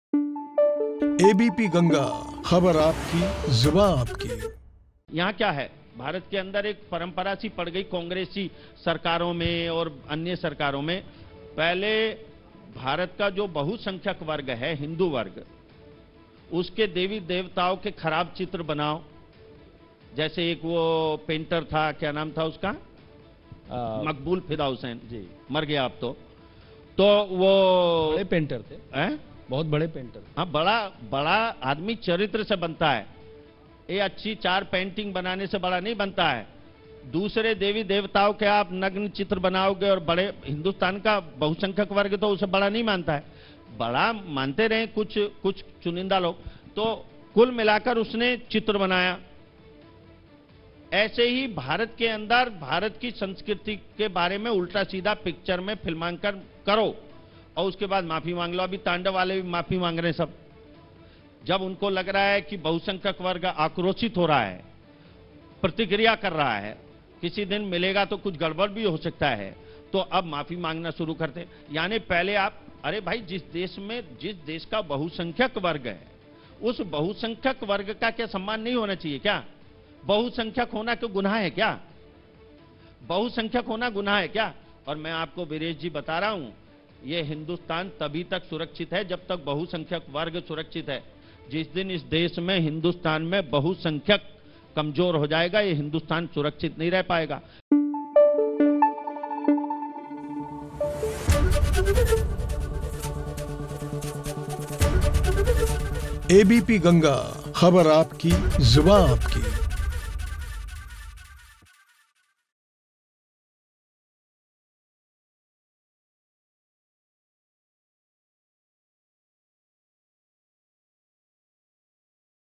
'हिंदुस्तान में बहुसंख्यक होना गुनाह है क्या ?' | Ashok Kataria
GangaMahaPanchayat में यूपी के मंत्री अशोक कटारिया ने बहुसंख्यकों को लेकर चिंता जाहिर करते हुए कहा कि क्या बहुसंख्यक होना गुनाह है. उनका कहना है कि जब तक इस देश में बहुसंख्यक सुरक्षित हैं. तब तक हिंदुस्तान सुरक्षित है.